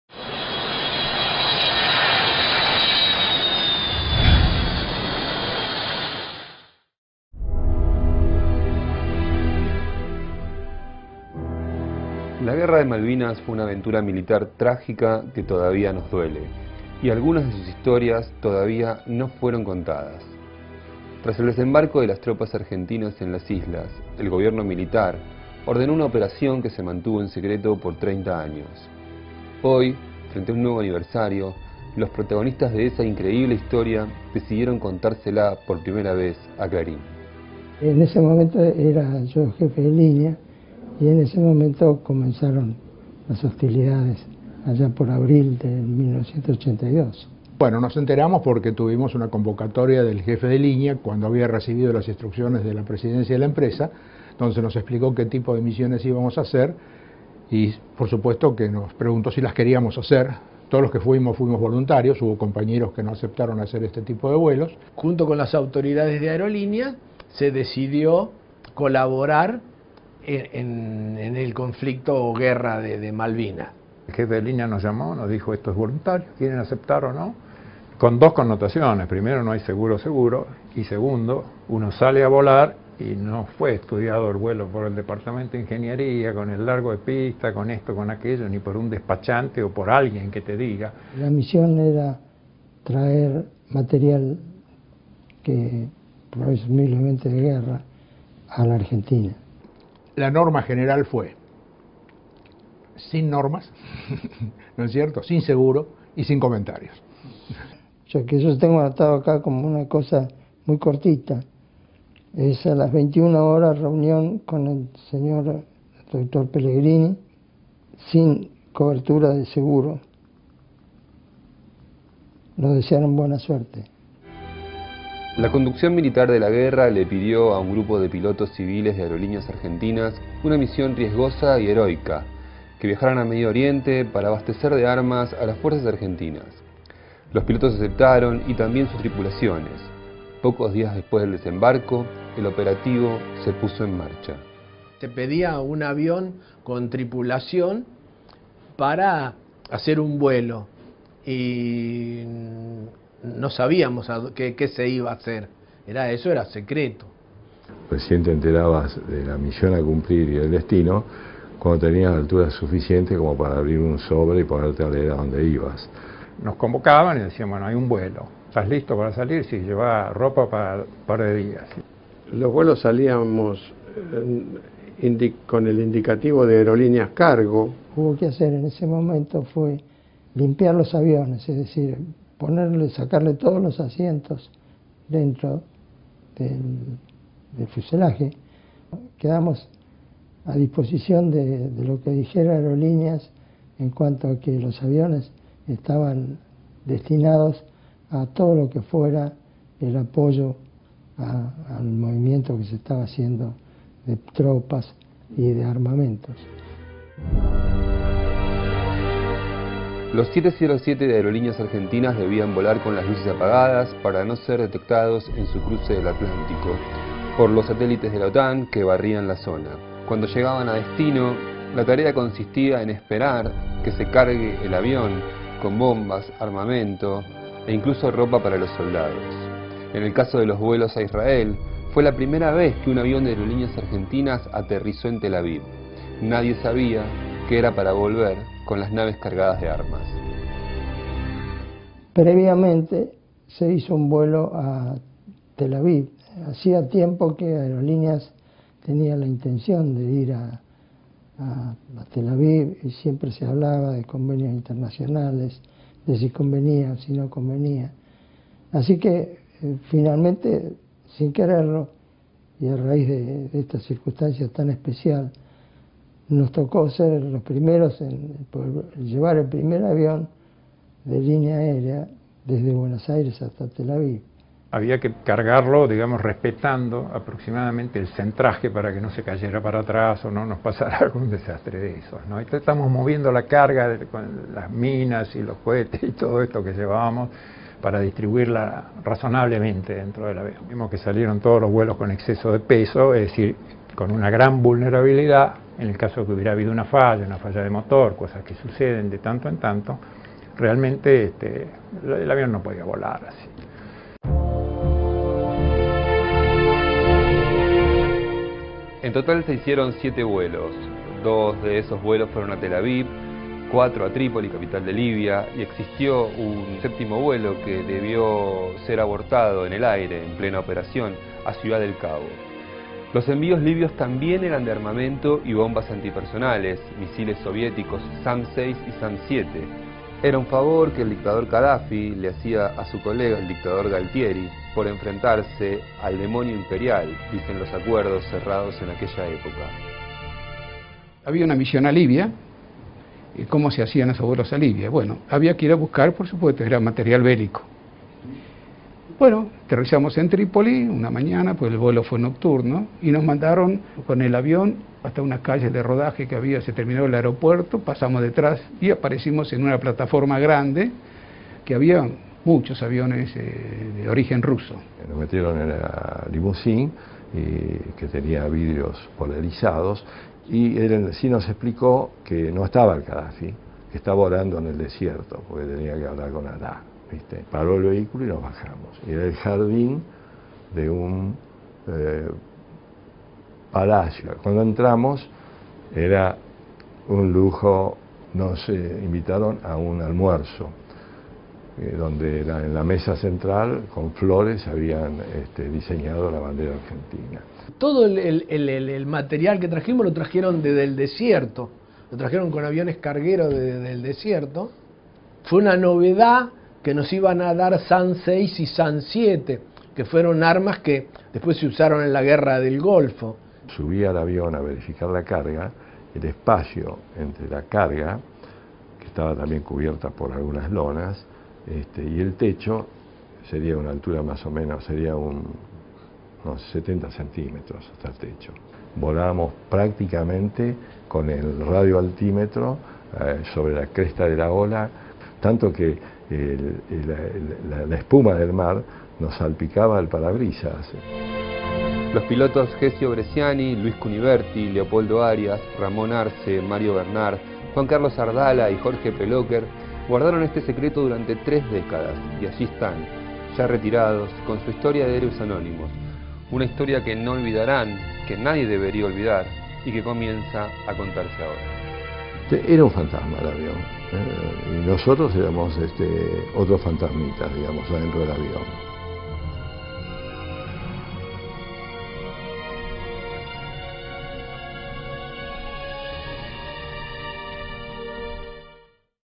Operacion Aerolineas
Las misiones se originaban a través del jefe de la Primera Brigada Aérea de Palomar. Luego, sobre el filo de la partida, se informaba el destino al comandante designado, previa entrega de un sobre con 40 mil dólares para utilizar en caso de imprevistos. Las rutas eran Buenos Aires-Recife- Las Palmas-Tel Aviv o Trípoli. La consigna era volar con luces apagadas y en silencio de radio. Sólo media hora antes de cada aterrizaje, los pilotos abrían la frecuencia para pedir las autorizaciones correspondientes: iban a buscar armamento para la guerra.
Así, durante casi dos meses, entre abril y junio de 1982, una cuadrilla de empleados civiles de Aerolíneas Argentinas llevó adelante un operativo secreto para traer armas al país desde Israel y Libia . Siete pilotos que participaron de la misión lo cuentan ahora, a 30 años, por primera vez.